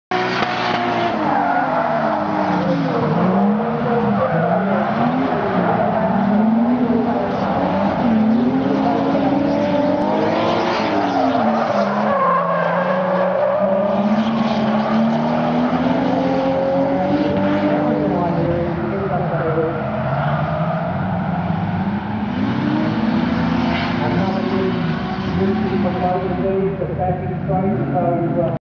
DRIFT RACELINE RED R32 DBLS GRN S13.5 PRO.avi
STADIUM DRIFT PRO TOUR # 5
]RED R32 DBLS GRN S13.5 PRO.
CARNELL RACEWAY